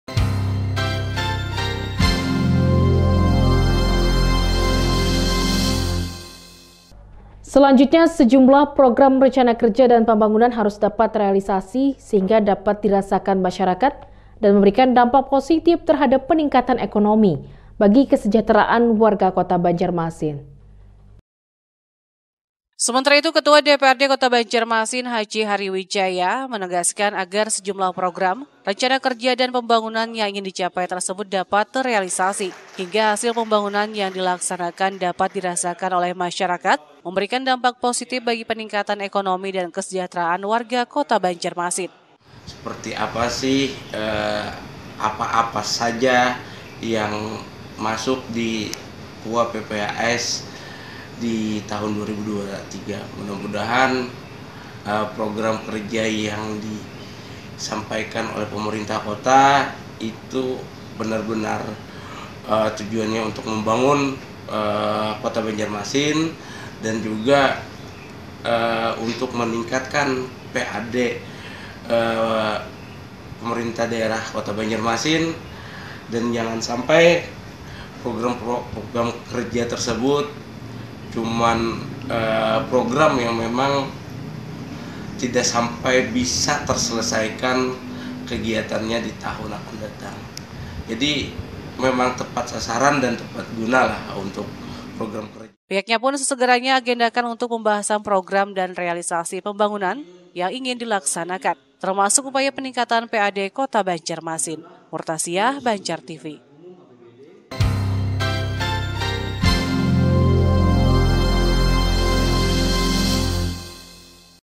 PROGRAM KERJA PEMBANGUNAN BISA DIREALISASIKAN
Sejumlah program rencana kerja dan pembangunan harus dapat terealisasi, sehingga dapat dirasakan masyarakat dan memberikan dampak positif terhadap peningkatan ekonomi bagi kesejahteraan Warga Kota Banjarmasin.